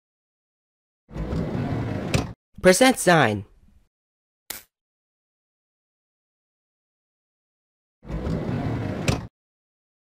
Percent Sign | Unicode Lore (episode 6)
"Dollar Sign went to the lobby while Hashtag and Quotation Mark went to gather the others."

I didn't get much done since I wanted to do other things, but I plan on uploading at least 1 episode daily.

Alphabet Lore is property of @MikeSalcedo.

If you're new here, please read my channel's "About" page.